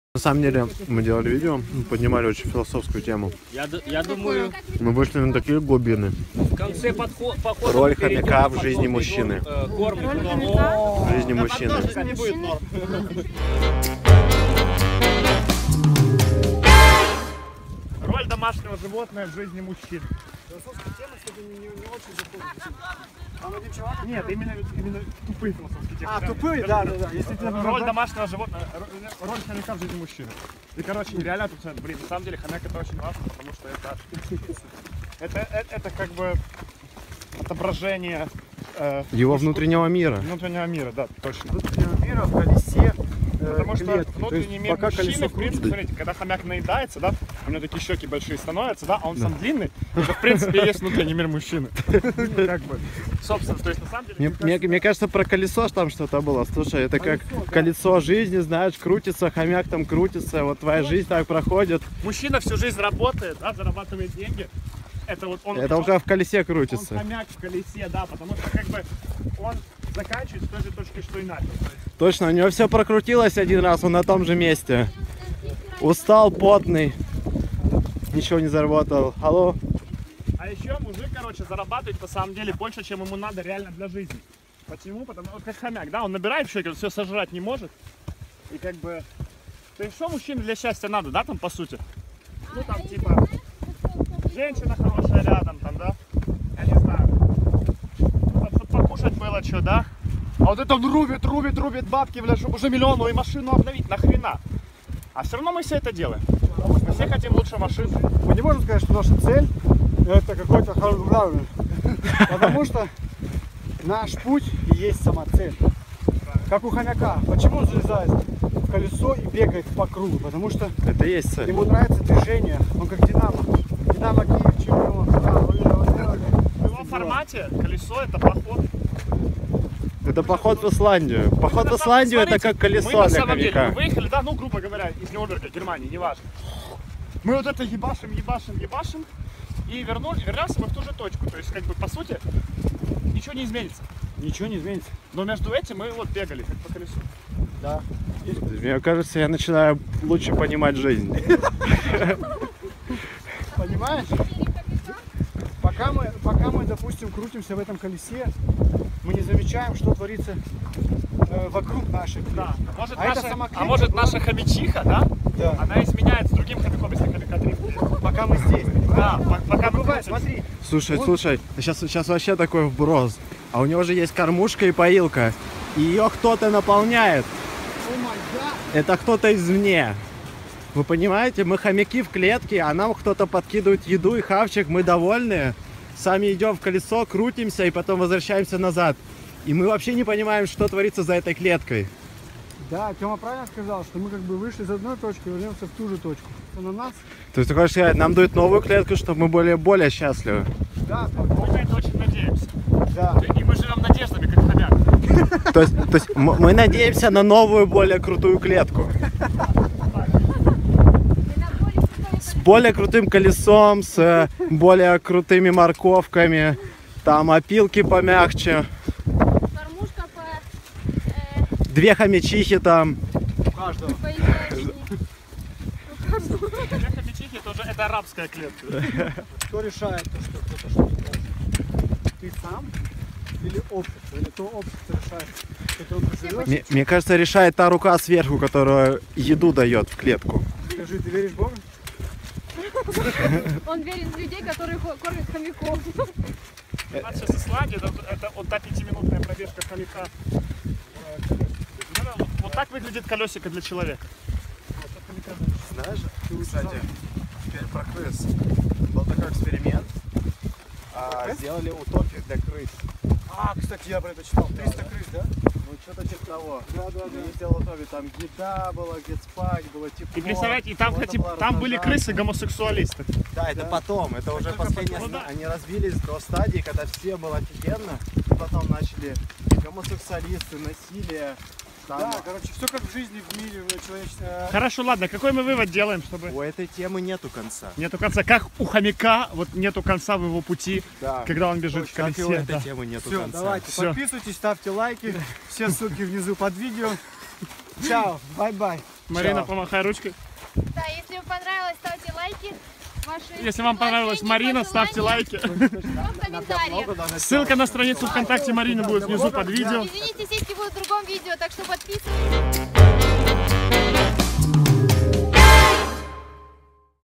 Роль хомяка в жизни мужчины
Горы и свежий воздух  Исландии открывают пути к свободному философскому мышлению.
Никто и не догадывался, а роль хомяка очень важна и глубокомысленна в жизни мужчины.